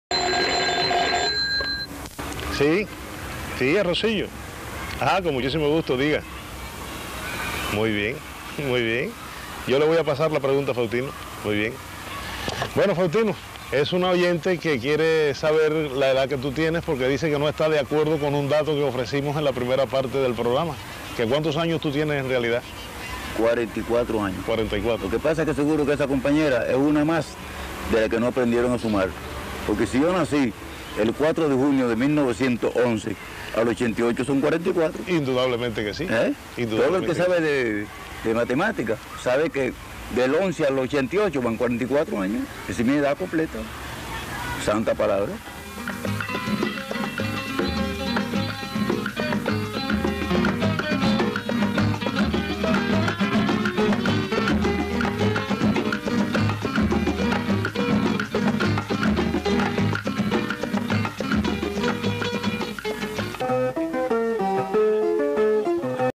Faustino Oramas - Eduardo Rosillo - Que Edad Tienes Faustino
♫ ♪. . . La Casa Del Rítmo y estos simpaticos momentos del recuerdo en que el gran Eduardo Rosillo con esa forma tan especial de dialogar, pregunta su edad a Faustino..._ Fautino, que edad tu tieneas_- a lo que el Juglar Holguinero responre con si colosal picardia. .que lo disfruten...♫ ♪ ..